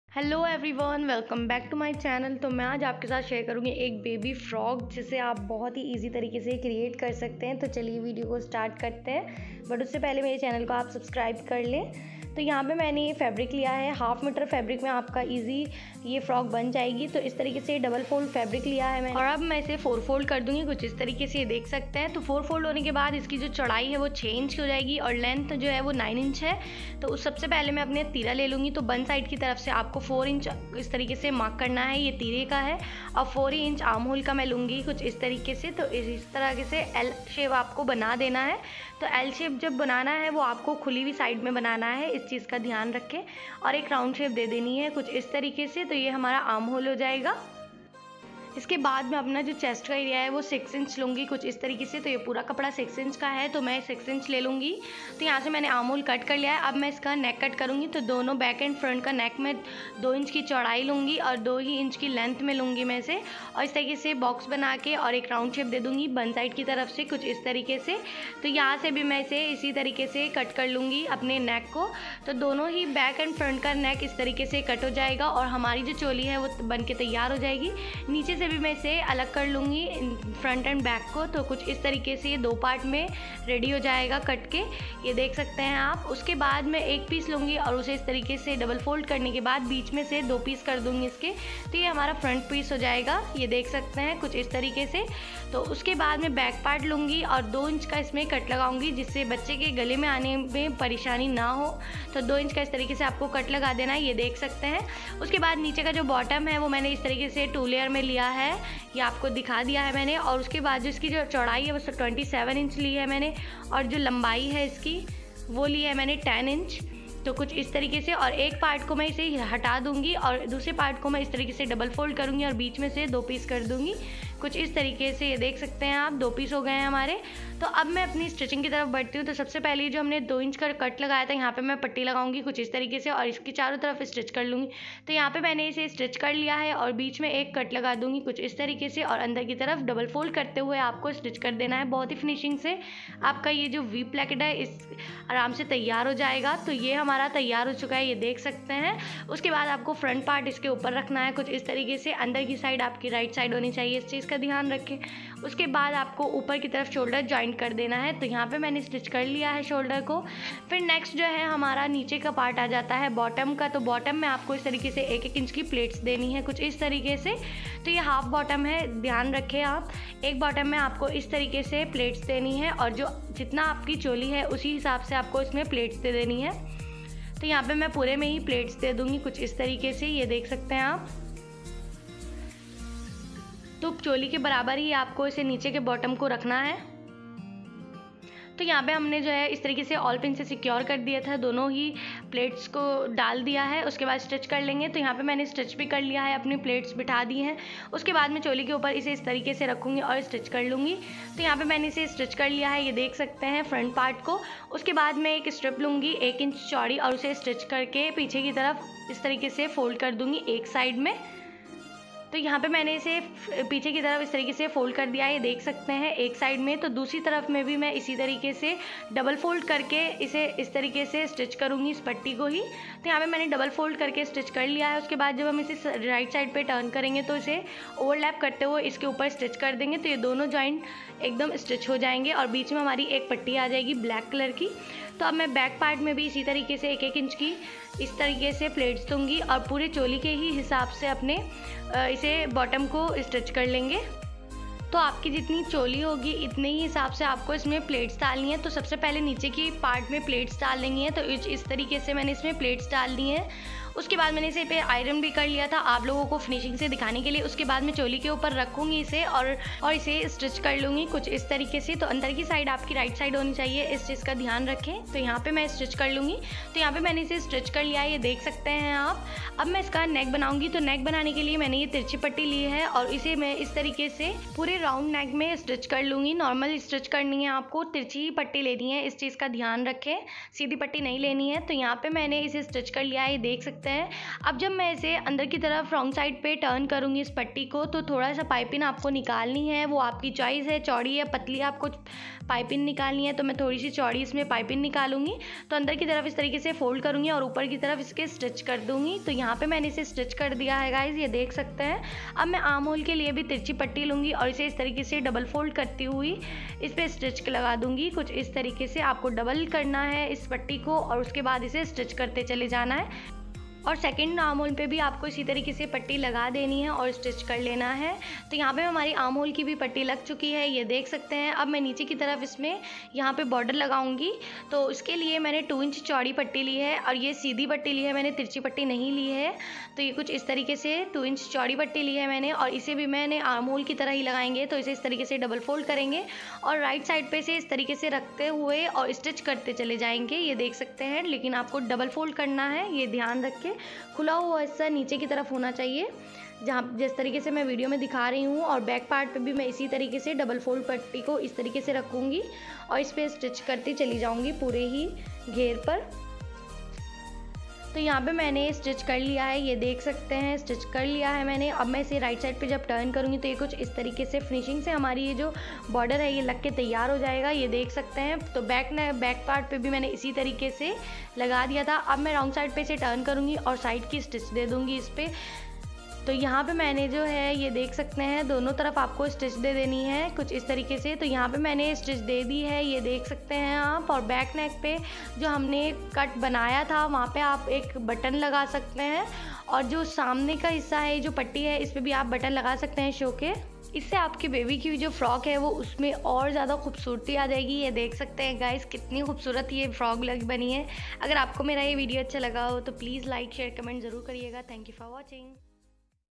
baby frock cutting and stitching easy tutorial||6 month baby frock cutting & stitching
hello everyone is video baby frock ki cutting and stitching btayi he

I hope apko meri video pasand aayegi

Or comant karke mujhe btaye ke kesa laga mera design.

Fr DD ghggt

सबसे आसानी से बनने वाली बारीक डोरी बनाना सीखें /how to make thin dorii by my fashion art